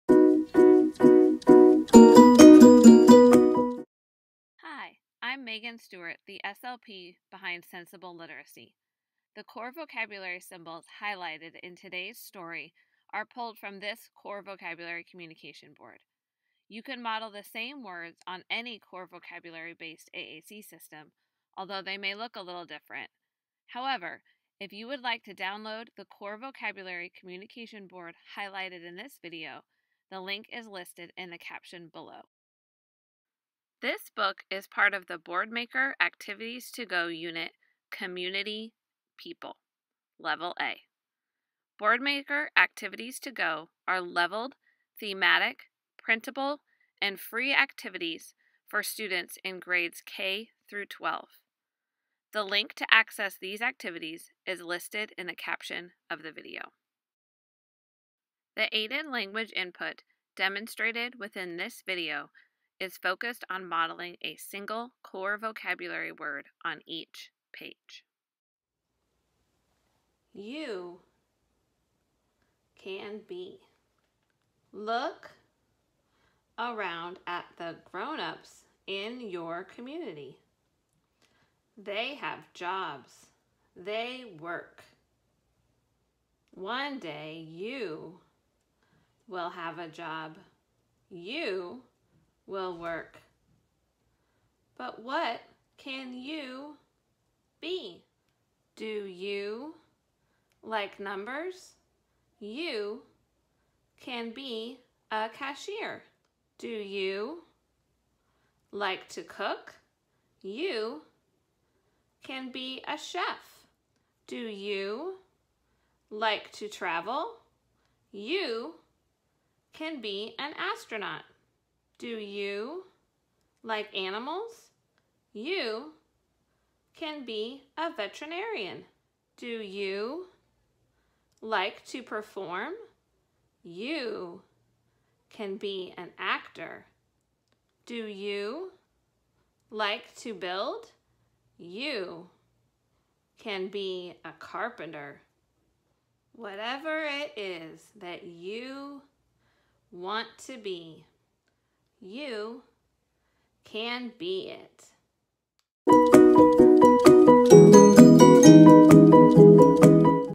Community Helpers: Modeling Core Vocabulary With AAC
Enjoy an interactive reading of the book "You Can Be" from Boardmaker Activities to Go Unit: Community (People) - Level A while modeling of a single core vocabulary word on each page is demonstrated.

Download the leveled, thematic, printable and free Boardmaker Activities to Go Units

Download the core vocabulary communication board highlighted within this video